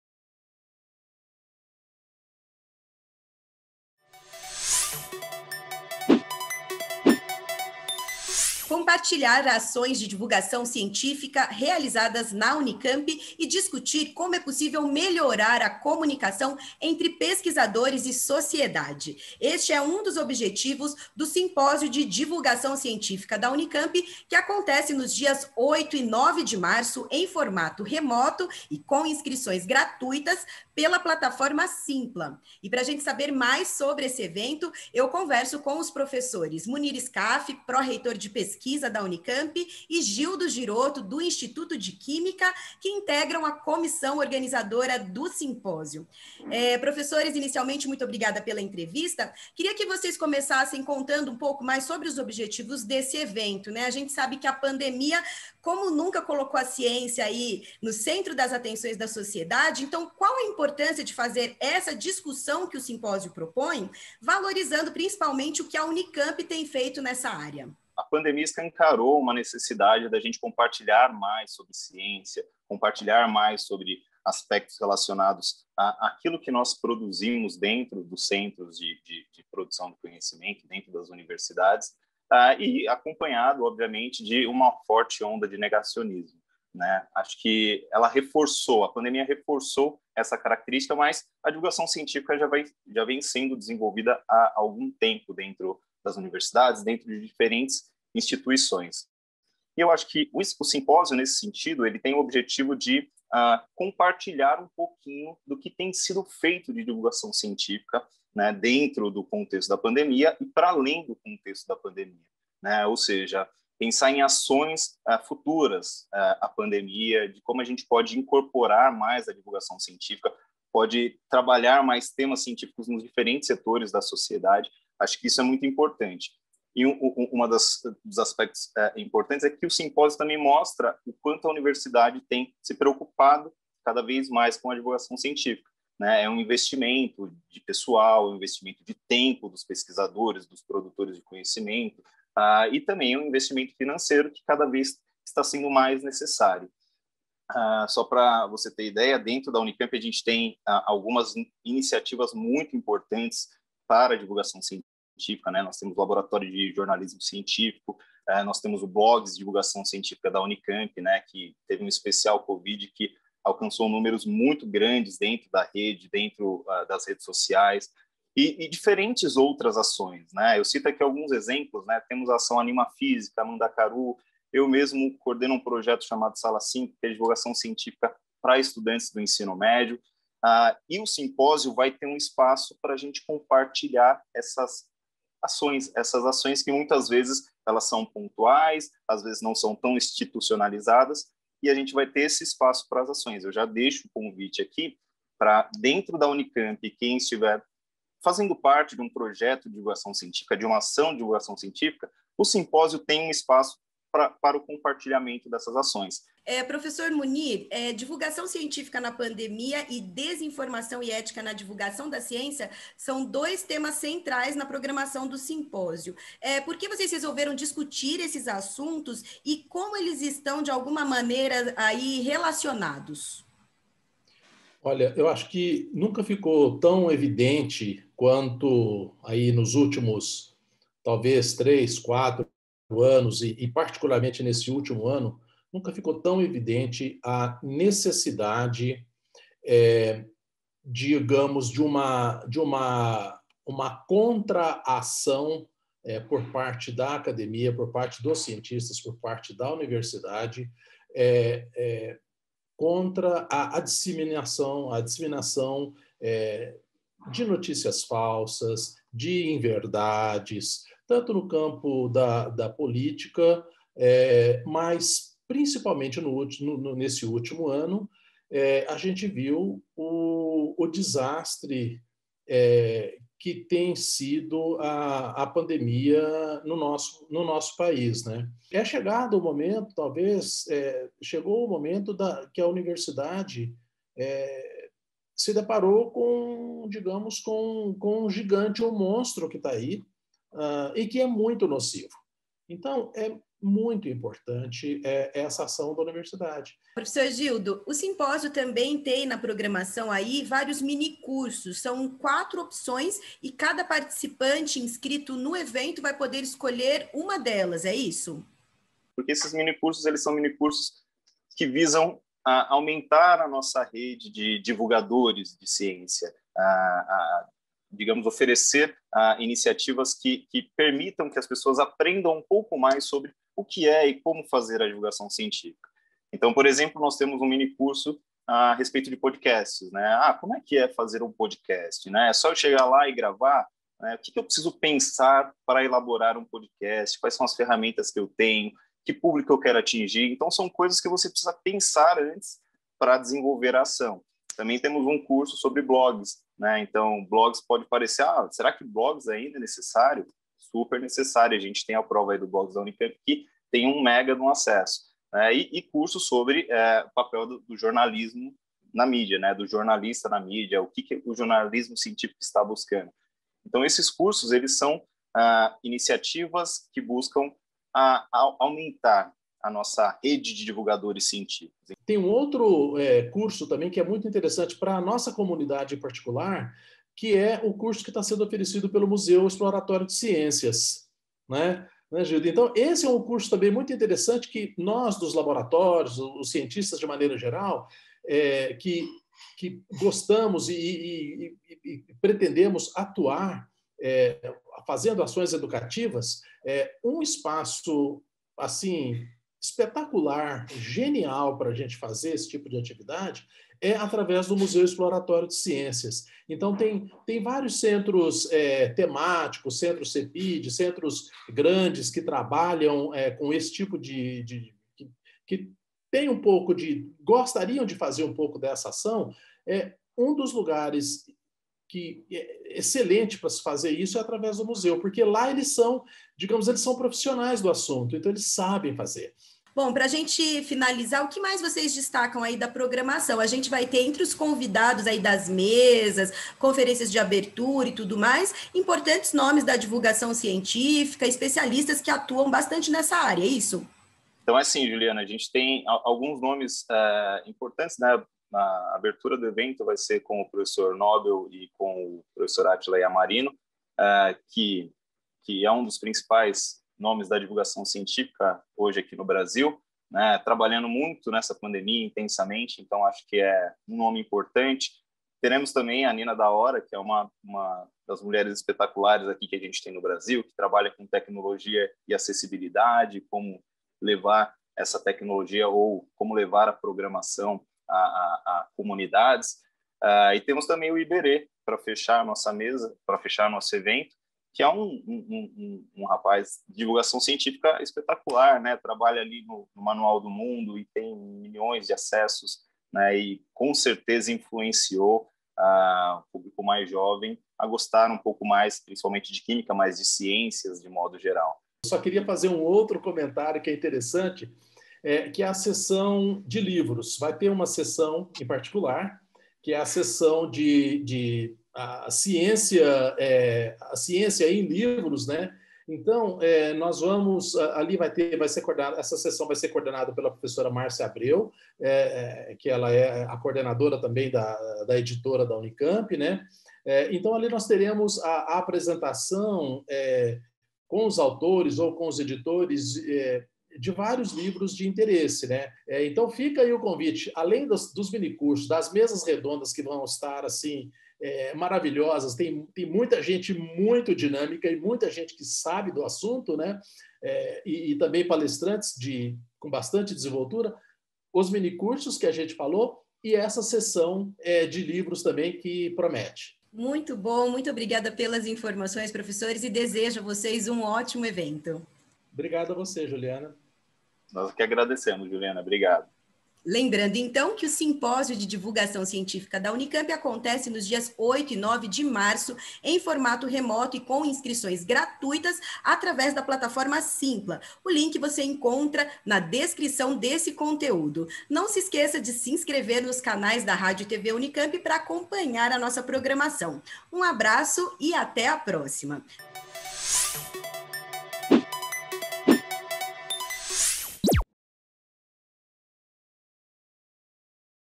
Unicamp reúne importantes nomes da divulgação científica em evento gratuito
Átila Iamarino, Nina da Hora e Iberê Thenório são apenas alguns dos destaques entre os convidados do Simpósio de Divulgação Científica da Unicamp, que acontece nos dias 8 e 9 de março, com inscrições abertas e gratuitas.

A proposta do evento é dar visibilidade a ações de divulgação científica realizadas na Unicamp, e também discutir como é possível melhorar a comunicação entre pesquisadores e sociedade, especialmente no atual cenário em que a ciência é desafiada a oferecer respostas cada vez mais precisas e enérgicas para o enfrentamento não só da pandemia em curso, mas também da onda de desinformação e notícias falsas que se alastra no Brasil e no mundo.

Nesta edição, os professores Munir Skaf, pró-reitor de Pesquisa, e Gildo Girotto, do Instituto de Química da Unicamp, dão mais detalhes sobre a programação do Simpósio, que conta com palestras, mesas-redondas, minicursos e lançamento de livros. Vale ressaltar que, até o dia 5 de março, a comunidade acadêmica pode enviar vídeos de até 2 minutos contando sobre suas ações de divulgação científica, para serem exibidos ao longo do evento.